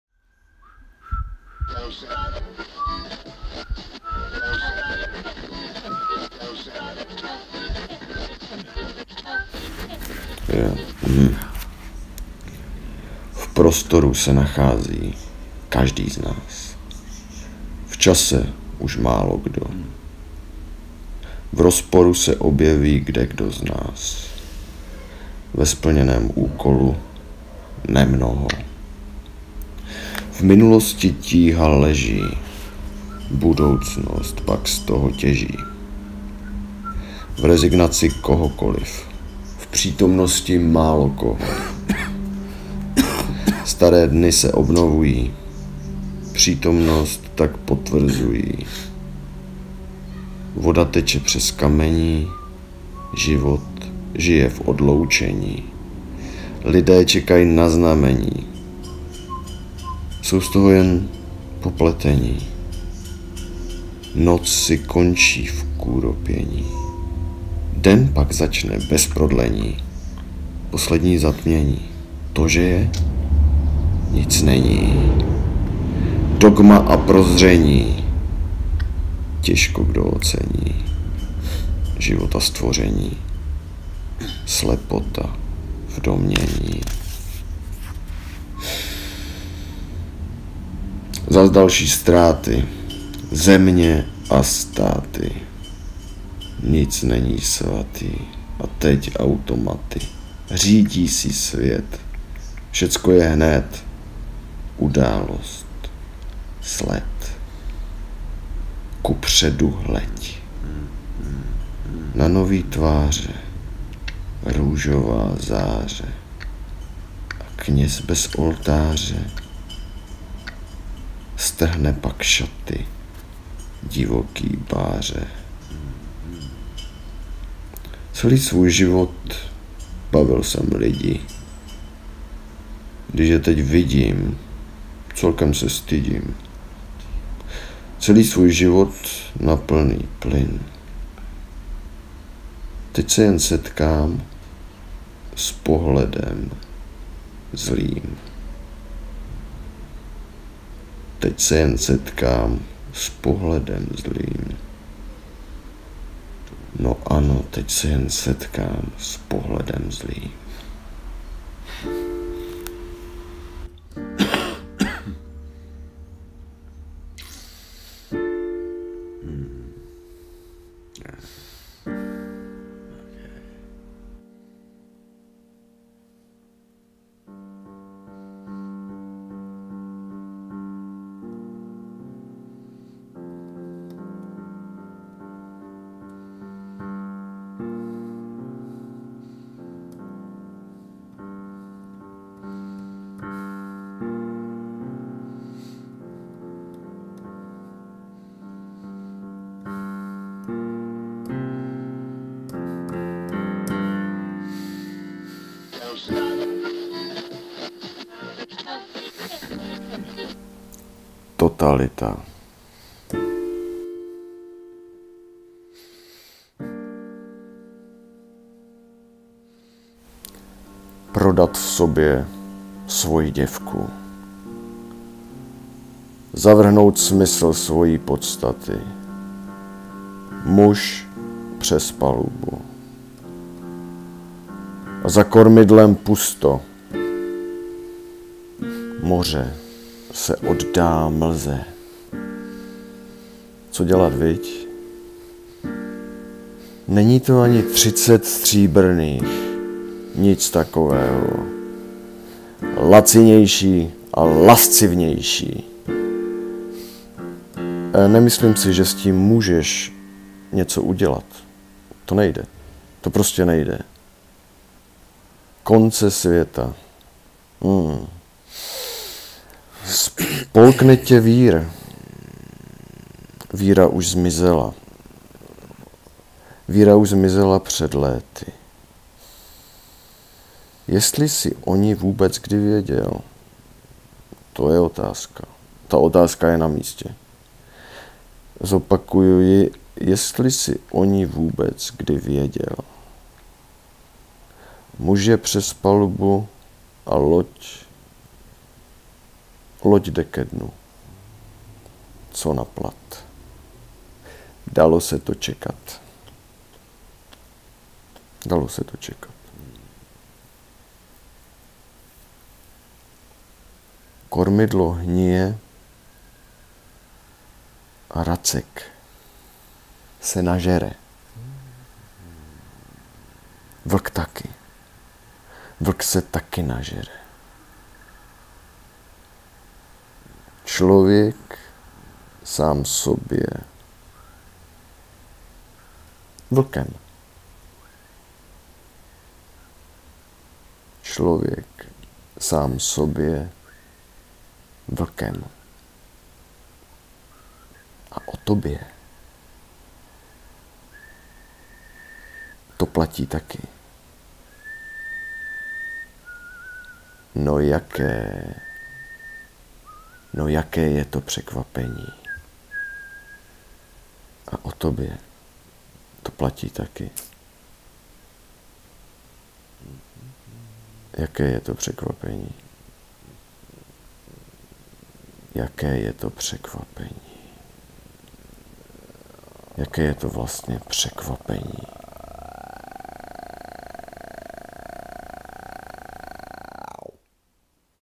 Patrik Šíma - Pohledem zlým... / Totalita (báseň)
Mé knihy (pod pseudonymem Lopéz García)

Pohledem zlým... / Totalita (báseň)
Červenec 2022

Pohledem zlým...

v prostoru se nachází každý z nás
v čase už málokdo
v rozporu se objeví kdekdo z nás
ve splněném úkolu nemnoho
v minulosti tíha leží
budoucnost pak z toho těží
v rezignaci kohokoliv
v přítomnosti málokoho
staré dny se obnovují
přítomnost tak potvrzují
voda teče přes kamení
život žije v odloučení
lidé čekaj na znamení
jsou z toho jen popletení
noc si končí v kuropění
den pak začne bez prodlení
poslední zatmění
to že je? - nic není
dogma a prozření
těžko kdo ocení
života stvoření
slepota v domnění
zas další ztráty
země a státy
nic není svatý
a teď automaty
řídí si svět
všecko je hned
událost, sled
kupředu hleď
na nový tváře
růžová záře
kněz bez oltáře
strhne pak šaty divoký Báře
celý svůj život bavil jsem lidi
když je teď vidím, celkem se stydím
celý svůj život na plný plyn
teď se jen setkám s pohledem zlým
no ano, teď se jen setkám s pohledem zlým

Totalita

prodat v sobě svoji děvku
zavrhnout smysl svojí podstaty
muž přes palubu
a za kormidlem pusto
moře se oddá mlze
co dělat, viď?
není to ani 30 stříbrných
nic takového
lacinější a lascivnější
nemyslím si, že s tím můžeš něco udělat - to nejde
to prostě nejde...
konce světa
spolkne tě vír
víra už zmizela,  víra už zmizela před léty
jestli jsi o ní vůbec kdy věděl
to je otázka
ta otázka je na místě
zopakuju ji
muž je přes palubu a loď... loď jde ke dnu
co naplat
dalo se to čekat
kormidlo hnije a racek se nažere
vlk taky
vlk se taky nažere
člověk sám sobě vlkem
a o tobě to platí taky
no jaké... no jaké je to překvapení?!
jaké je to překvapení?
jaké je to vlastně překvapení???